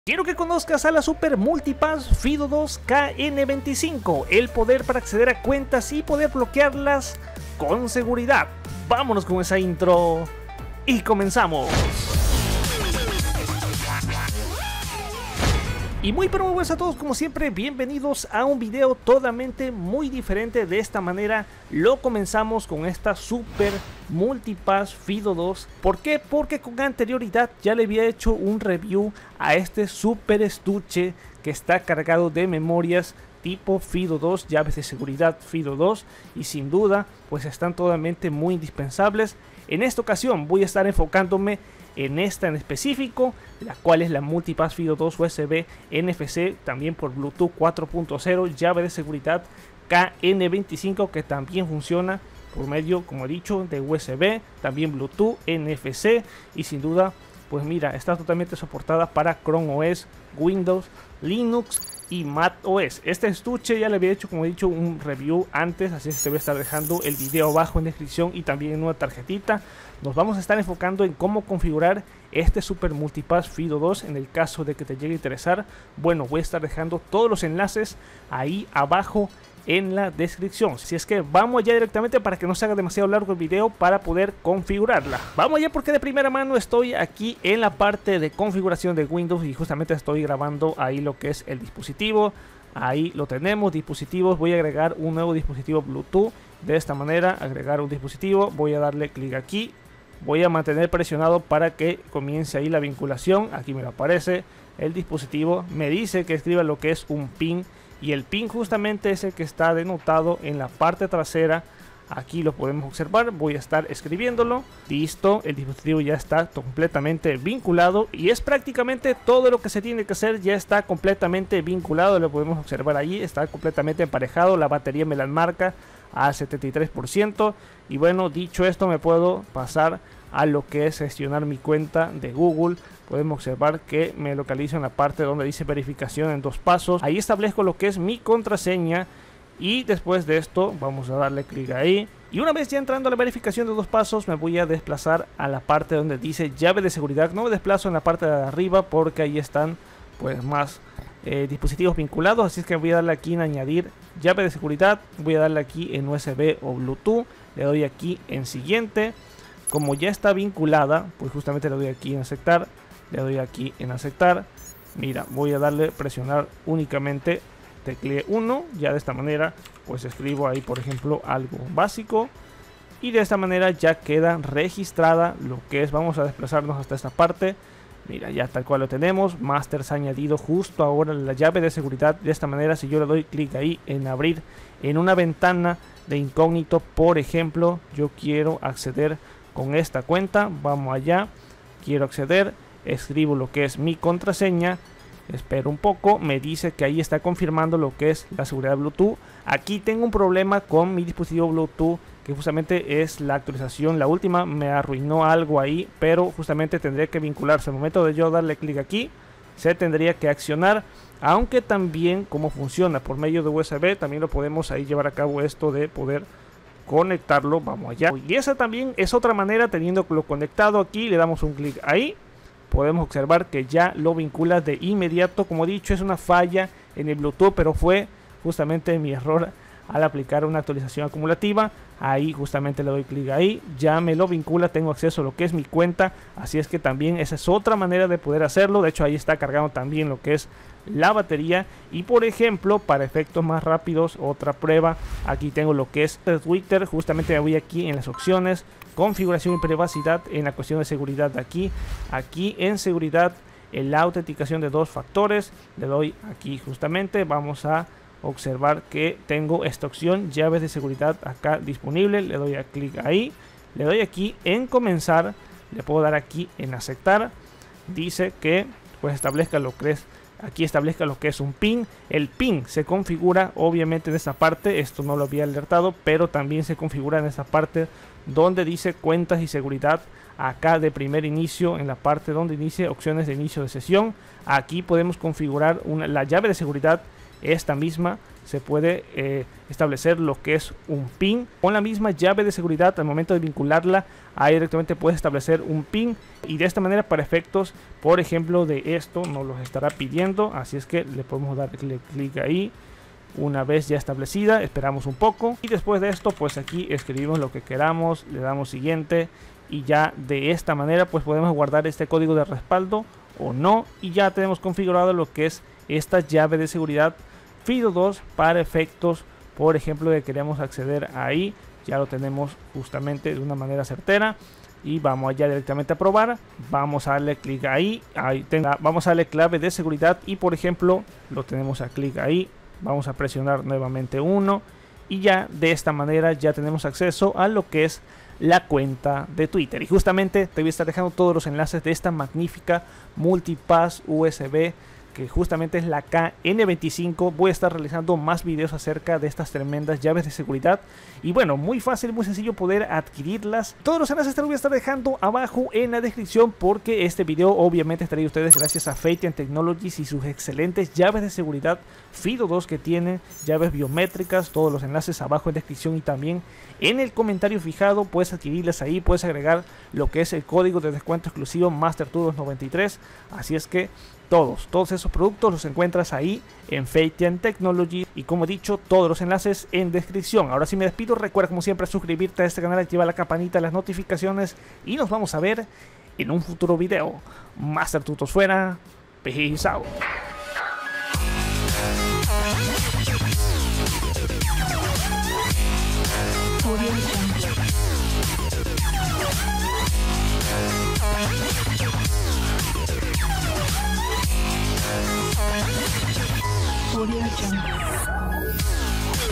⭐NUEVA! Llave FIDO 2 KN25 / BLOQUEA y PROTEGE tus CUENTAS RAPIDAMENTE 2021
⭐FEITIAN proporciona sólidas soluciones de autenticación para satisfacer las necesidades de los usuarios, proporciona una gran cantidad de herramientas de seguridad y sin duda vale la pena conocer todos los sensacionales dispositivos con lo que cuenta.

🌀Te comparto los enlaces de las LLAVES USB destacas en el video.🌀

●▬▬▬▬๑۩۩๑▬▬▬▬▬●

●▬▬▬▬๑۩۩๑▬▬▬▬▬●

sεяα dεnunciαdα.